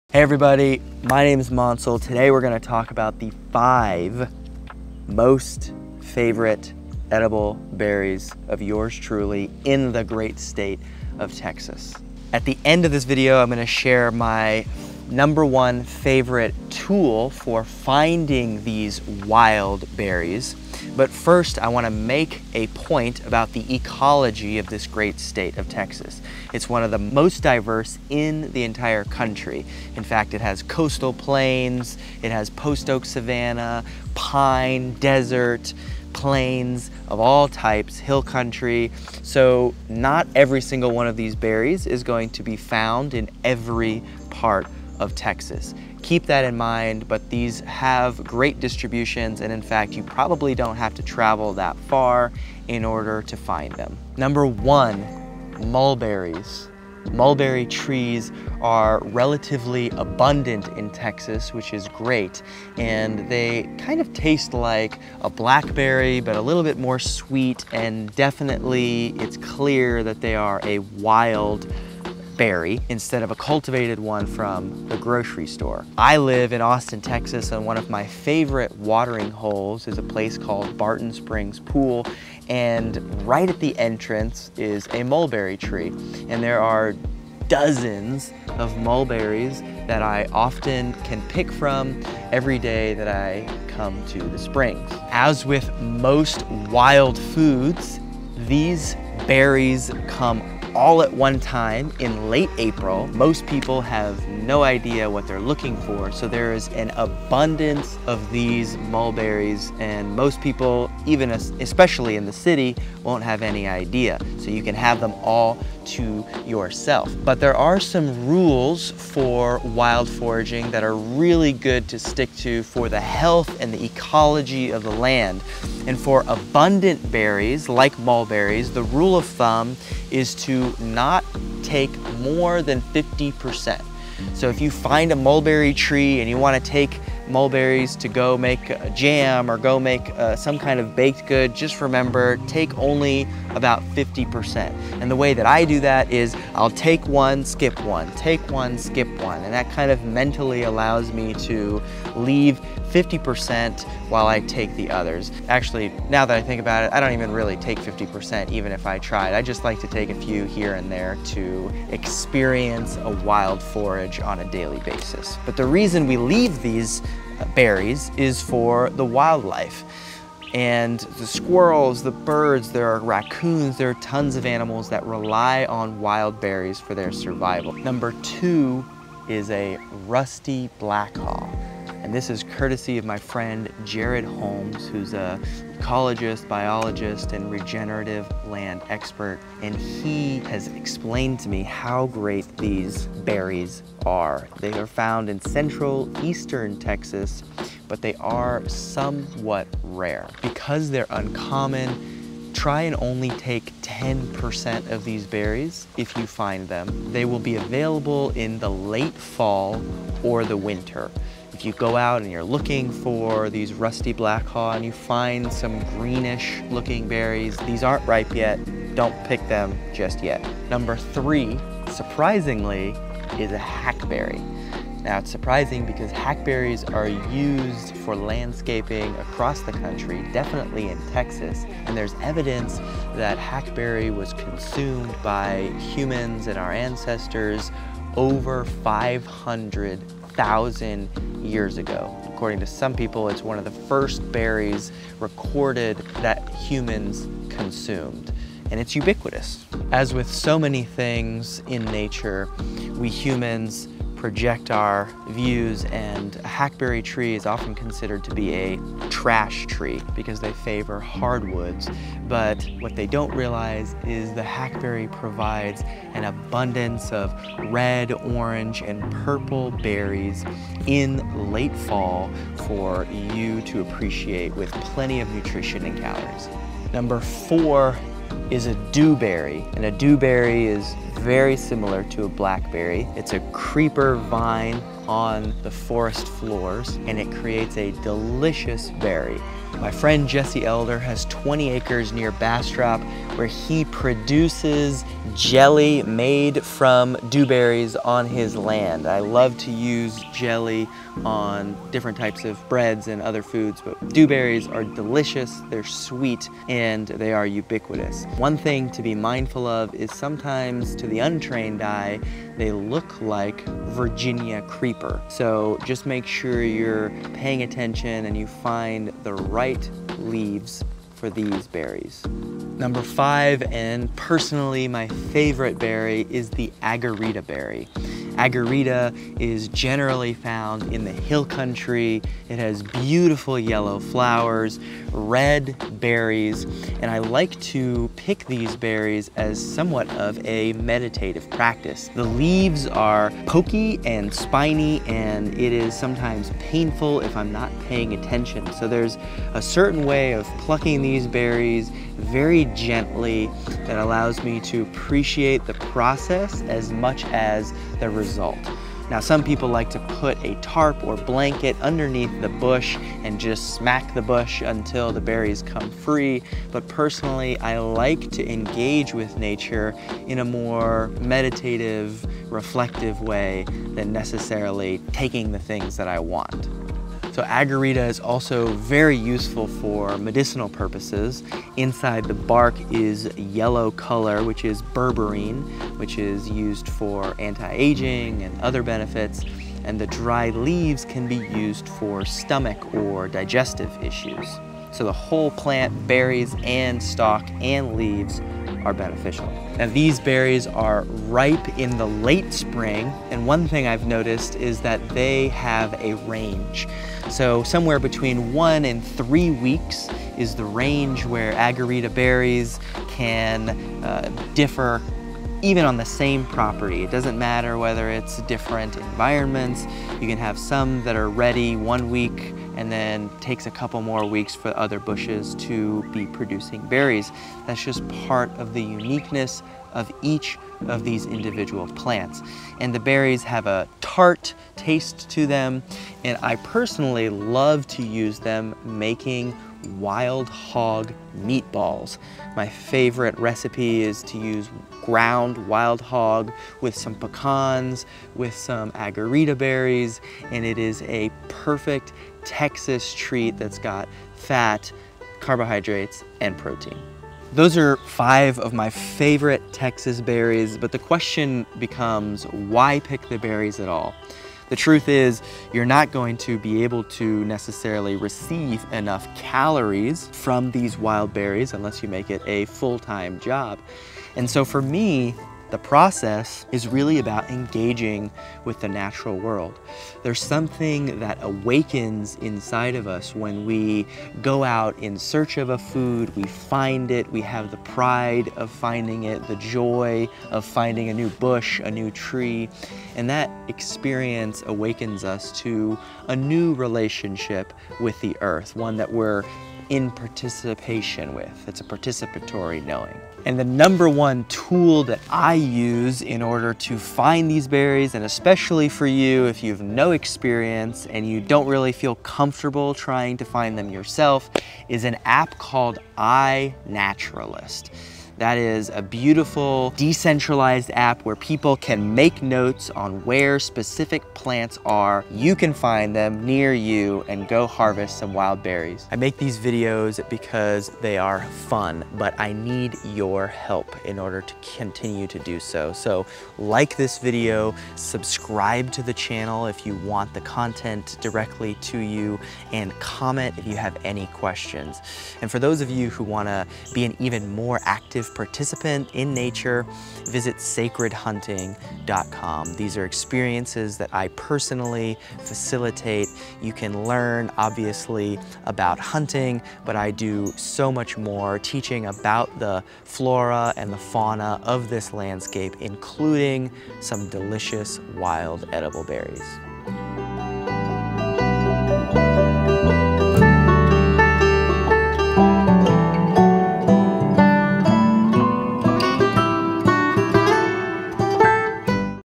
Top 5 Wild Edible Berries in Texas
Abundance surrounds us. Sometimes it is in the form of wild animals we can hunt, kill, and consume. Sometimes it is in the form of wild edible berries!

This list of wild edible berries in Texas includes a great mix of abundant and rarer options that will delight the tastebuds and excite the forager in each of us.

It’s possible that some of these berries are in such abundance you can cook with them, harvest them for numerous meals, or simply have a snack every time you visit.

Either way, enjoy this list of wild foraged berries that includes where to find them, when to find them, and all other details you need to make this a part of your food gathering plan!